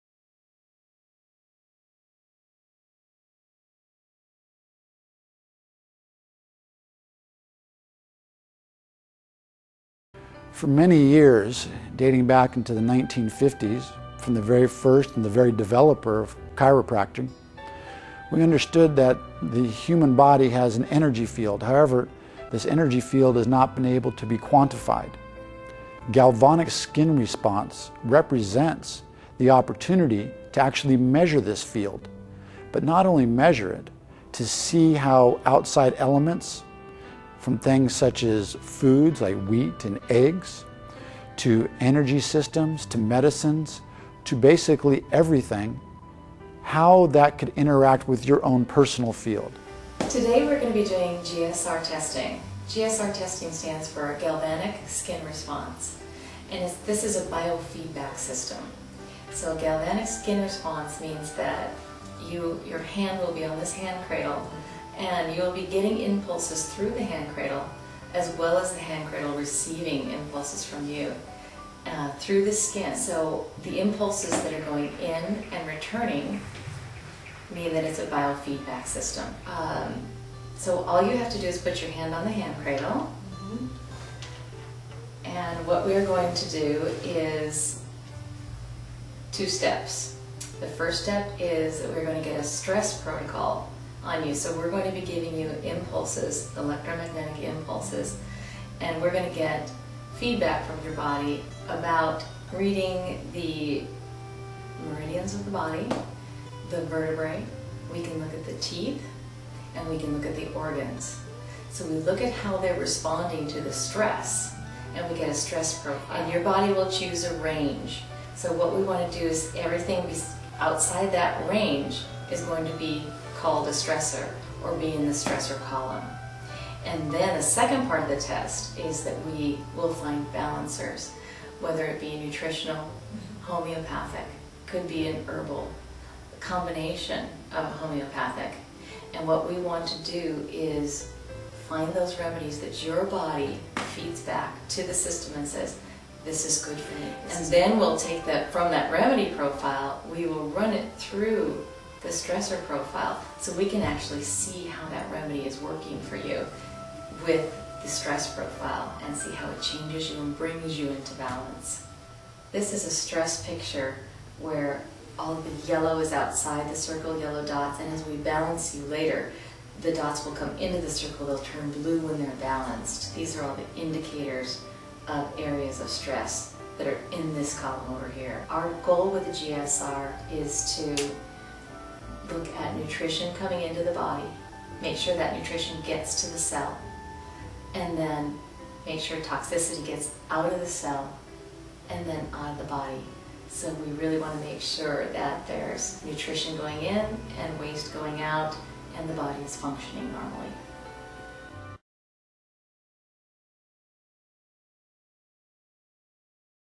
Galvanic Skin Response (G.S.R.) Testing
The body is comprised of various systems that respond to stimuli. The smell of food (stimulus) triggers the mouth to salivate (response). A loud noise (stimulus) triggers the release of adrenaline into the blood and an increased heart rate (response). There are various ways to measure the body's response to stimuli, including GSR or Galvanic Skin Response. GSR measures the conductivity (how well something conducts electricity) of the largest organ in the body, the skin. The conductivity of the skin was first discovered around 1888 by Tarchanoff. In the early 1900s, Dr. Carl Jung found that GSR measurements could track physiological arousal or stress in the body. In the 1930s, Dr. Hans Selye began to uncover the importance of understanding stress and its impact upon the different systems of the body. Discovering the importance of stress in the body, its effect on our health, and its ability to be measured in the body through GSR led to the creation of many common devices, such as the polygraph test.